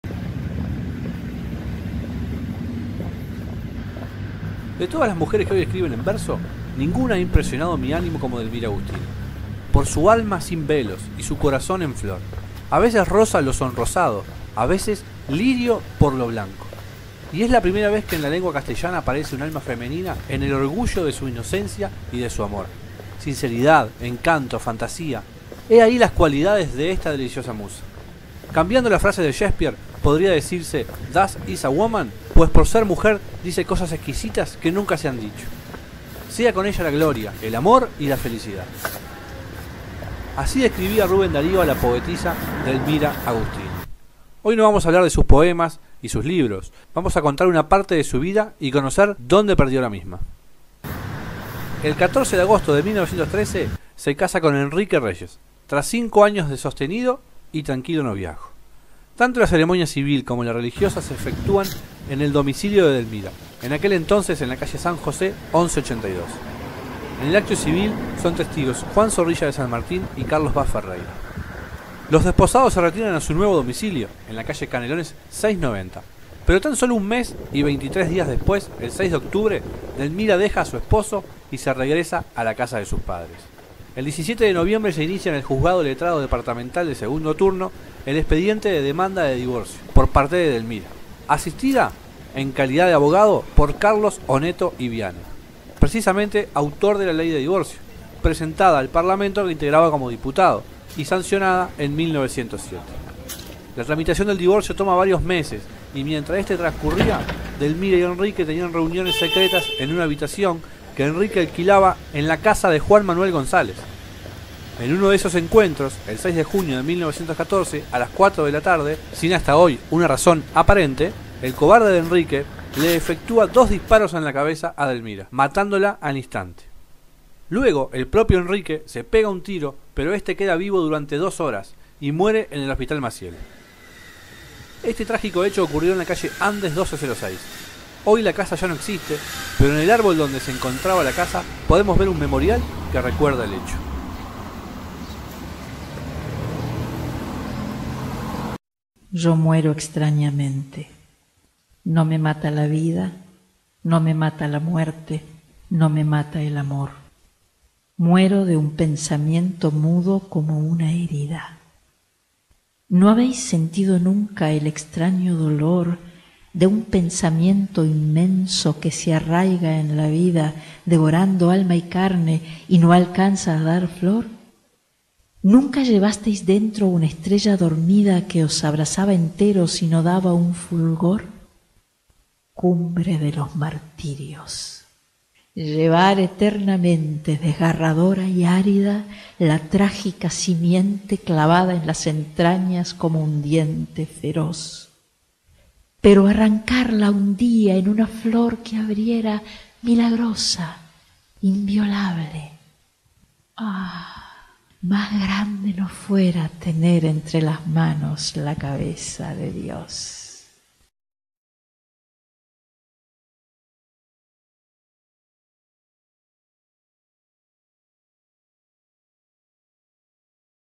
Caminando por Montevideo - Delmira Agustini
Hoy la caminata por Montevideo nos va trasladas hasta el lugar donde la poeta uruguaya Delmira Agustini perdió la vida.

El poema "Lo inefable" de Derlmira es interpretado por Raquel Diana.